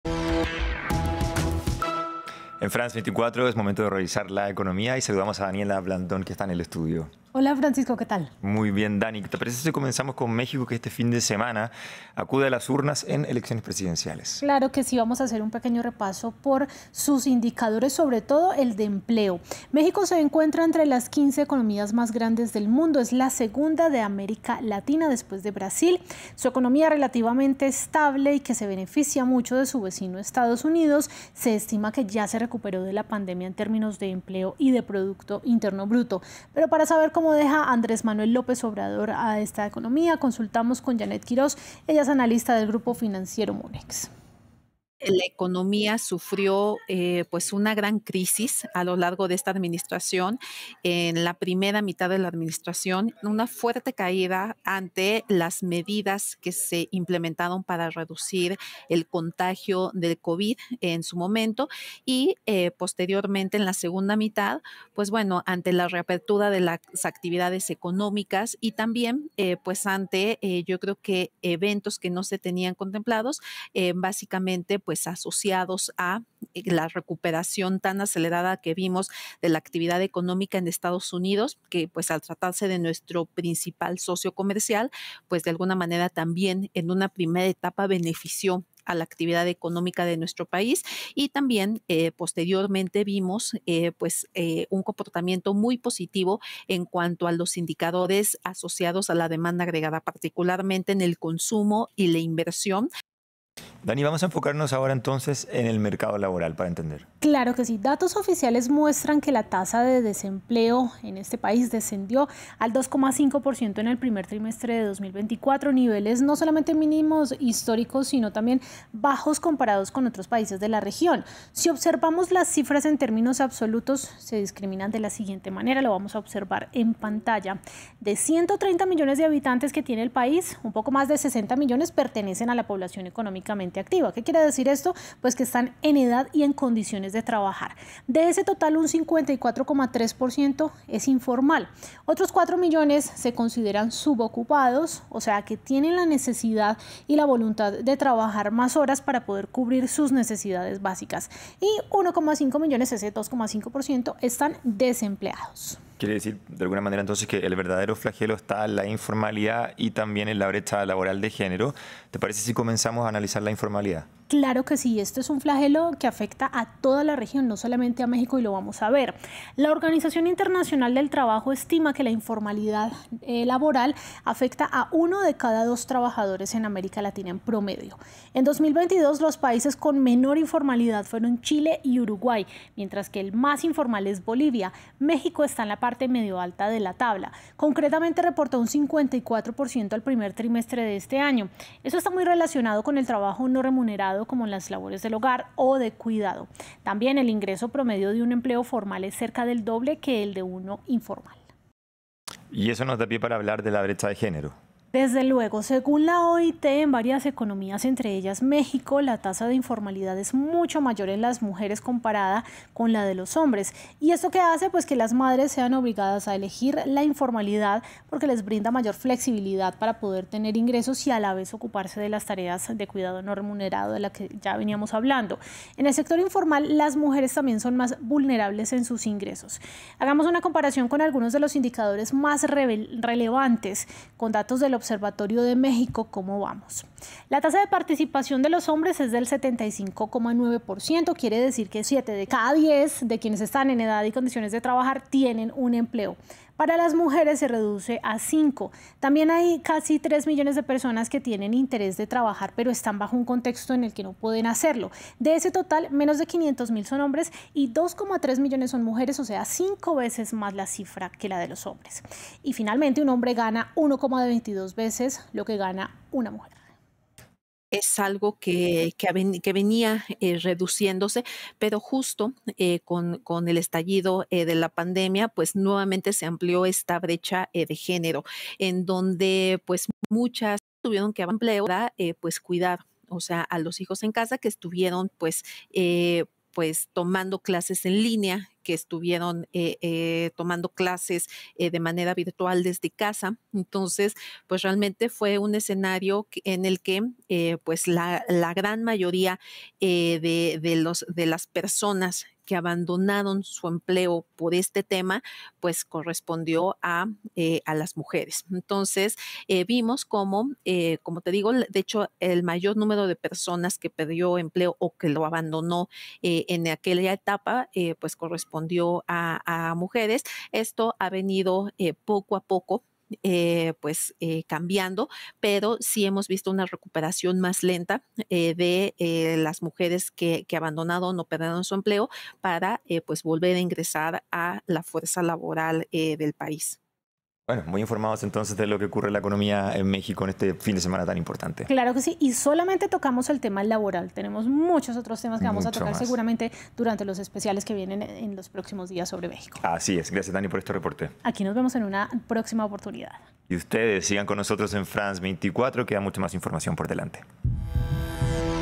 México en cifras: la economía que recibió y la que dejó Andrés Manuel López Obrador
El saliente presidente de México tiene varios logros por mostrar y, a su vez, varias tareas que le deja pendiente a su sucesor, sobre todo en lo que tiene que ver con la erradicación de la pobreza extrema. France 24 hace un repaso por los principales indicadores económicos de la gestión presidencial de los últimos seis años.